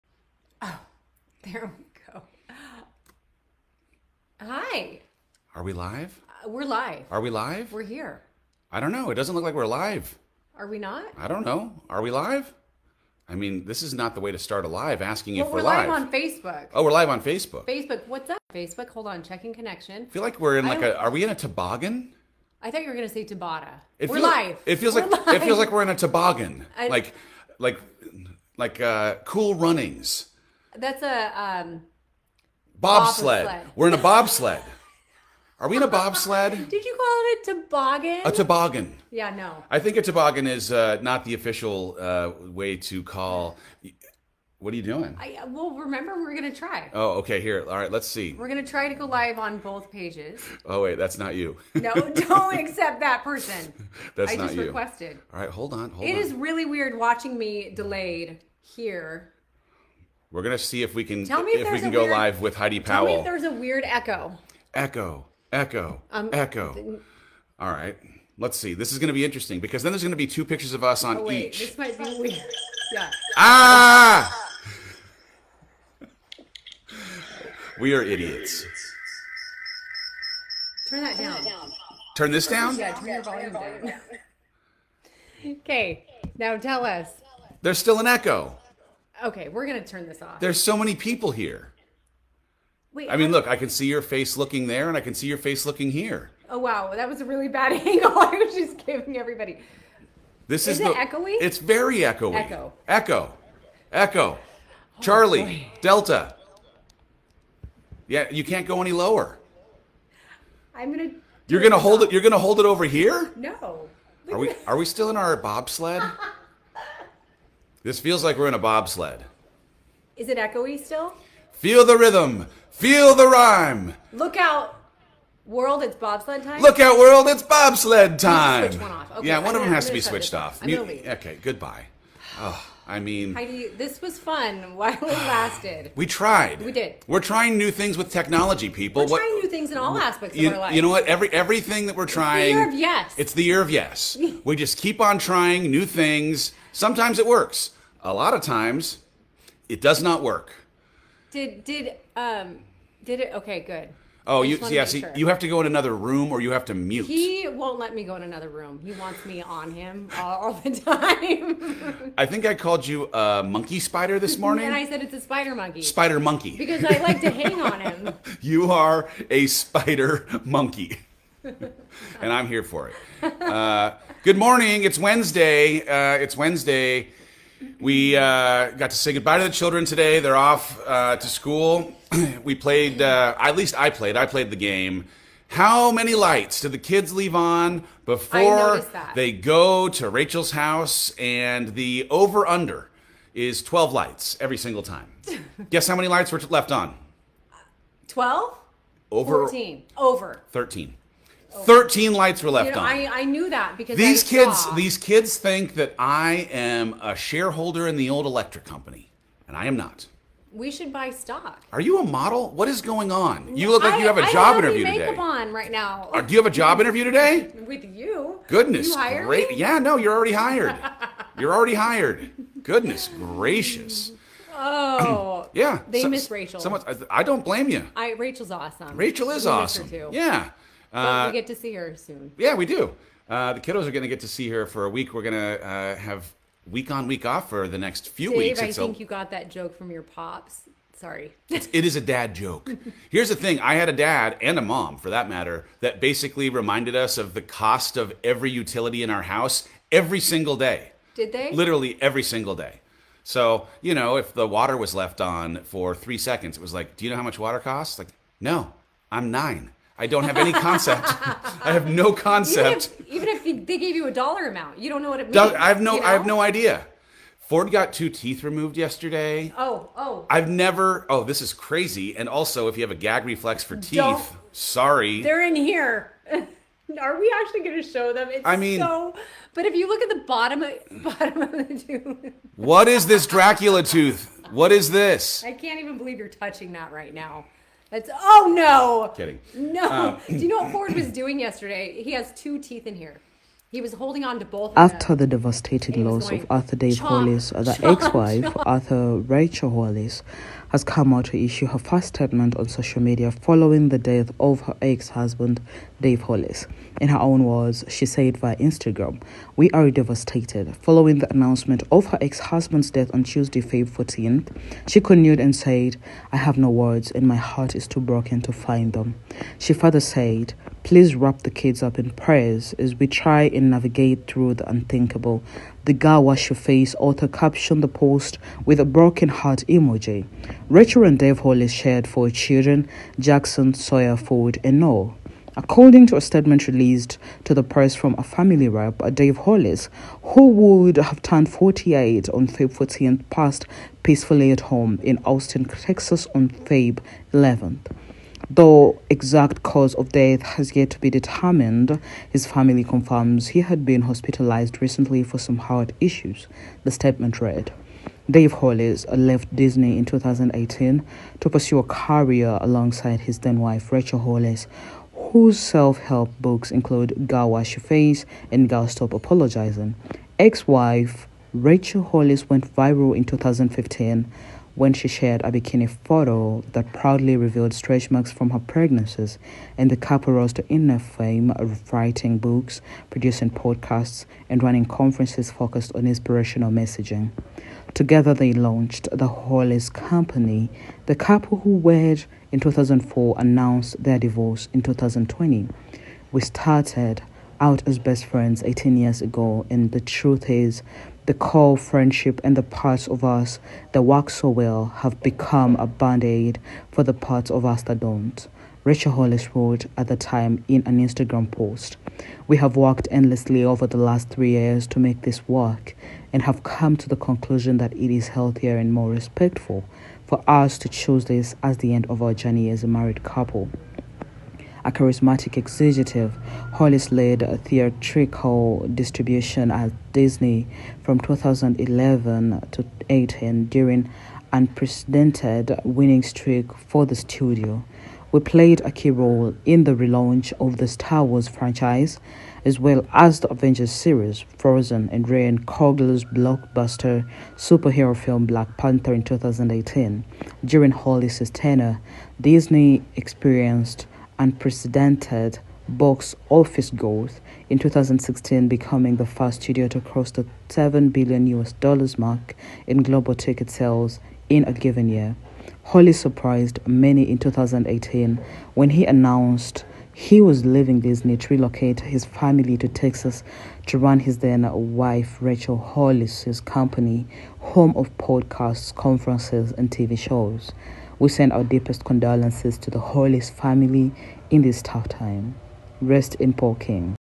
FUNERAL:Rachel Hollis Mourns Sudden Death Of Dave Hollis 😭
After the devastating loss of author Dave Hollis

The ex wife, Author Rachel Hollis has come out to issue her first statement on social media following the death of her ex-husband, Dave Hollis.

In her own words she said via Instagram

"We are devastated,"

following the announcement of her ex-husband's death on Tuesday, Feb. 14th

She continued and said I have no words and my heart is too broken to find them."

She further said , "Please wrap the kids up in prayers as we try and navigate through the unthinkable."

The “Girl, Wash Your Face” author captioned the post with a broken heart emoji.

Rachel and Dave Hollis share four children: Jackson, Sawyer, Ford and Noah.

According to a statement released to the press from a family rep, Dave Hollis, who would have turned 48 on Feb. 14, passed peacefully at home in Austin, Texas on Feb. 11.

"Though exact cause of death has yet to be determined, his family confirms he had been hospitalized recently for some heart issues," the statement read.

Dave Hollis left Disney in 2018 to pursue a career alongside his then-wife, Rachel Hollis, whose self-help books include “Girl, Wash Your Face” and “Girl, Stop Apologizing.”

Ex-wife Rachel Hollis went viral in 2015 when she shared a bikini photo that proudly revealed stretch marks from her pregnancies and the couple rose to internet fame writing books, producing podcasts and running conferences focused on inspirational messaging.

Together, they launched “The Hollis Company.”

 The couple, who wed in 2004, announced their divorce in 2020.

“We started out as best friends 18 years ago and the truth is, that core friendship and the parts of us that work so well, have become a band-aid for the parts of us that don’t," Rachel Hollis wrote at the time in an Instagram post. "

We have worked endlessly over the last three years to make this work and have come to the conclusion that it is healthier and more respectful for us to choose this as the end of our journey as a married couple.”

A charismatic executive, Hollis led theatrical distribution at Disney from 2011-18 during an unprecedented winning streak for the studio.

He played a key role in the relaunch of the Star Wars franchise, as well as the Avengers series, Frozen and Ryan Coogler’s blockbuster superhero film Black Panther in 2018).

During Hollis’ tenure, Disney experienced unprecedented box office growth, in 2016 becoming the first studio to cross the $7 billion mark in global ticket sales in a given year.

Hollis surprised many in 2018 when he announced he was leaving Disney to relocate his family to Texas to run his then-wife Rachel Hollis’ company, home of podcasts, conferences and TV shows.

Rest in power King